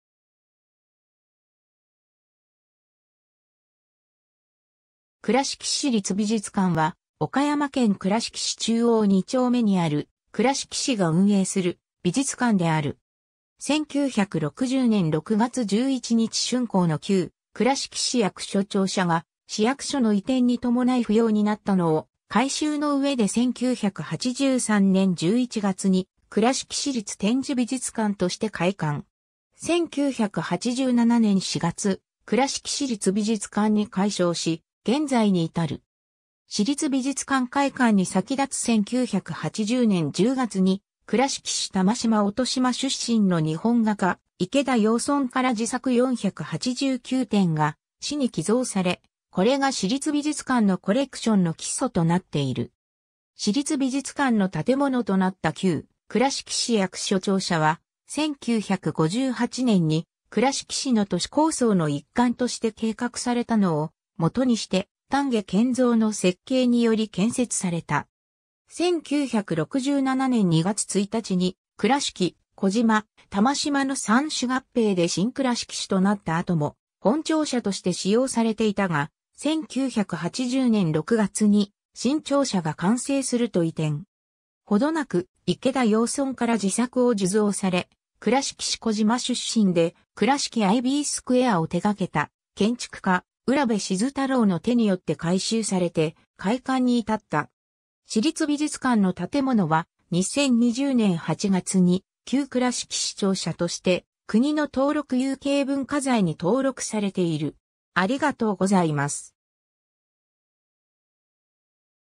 倉敷市立美術館
倉敷市立美術館（くらしきしりつびじゅつかん）は、岡山県倉敷市中央二丁目にある倉敷市が運営する美術館である。
 1960年（昭和35年）6月11日竣工の旧倉敷市役所庁舎が市役所の移転に伴い不要になったのを、改修の上で1983年（昭和58年）11月 に「倉敷市立展示美術館」として開館。
1987年（昭和62年）4月 「倉敷市立美術館」に改称し現在に至る。
市立美術館開館に先立つ1980年（昭和55年）10月 に倉敷市玉島乙島出身の日本画家池田遙邨から自作489点が市に寄贈され、これが市立美術館のコレクションの基礎となっている。
 市立美術館の建物となった旧倉敷市役所庁舎は、1958年（昭和33年）に倉敷市の都市構想の一環として計画されたのを基にして、丹下健三の設計により建設された。
1967年（昭和42年）2月1日 に倉敷・児島・玉島の三市合併で新倉敷市となった後も本庁舎として使用されていたが、1980年（昭和55年）6月 に新庁舎が完成すると移転。
程なく池田遙邨から自作を受贈され、倉敷市児島出身で倉敷アイビースクエアを手がけた建築家浦辺鎮太郎の手によって改修されて開館に至った。
 市立美術館の建物は、2020年（令和2年）8月に「旧倉敷市庁舎（倉敷市立美術館）」として国の登録有形文化財に登録されている。